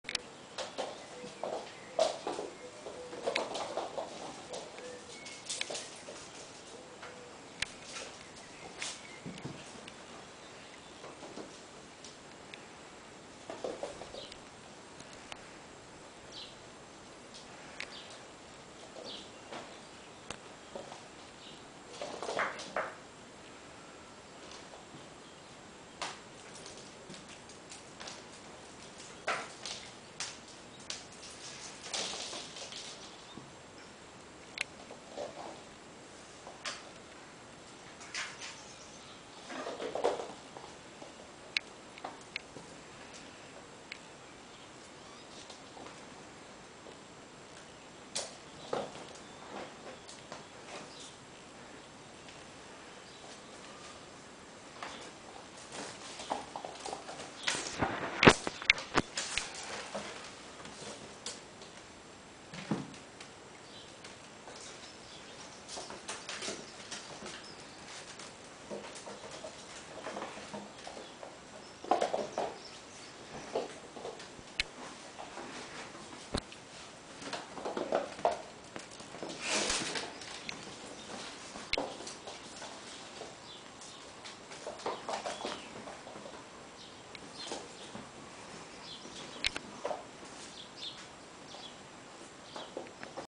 Sphynx kittens 11 weeks old playing
Sphynx kittens 3 black and 1 black/white playing.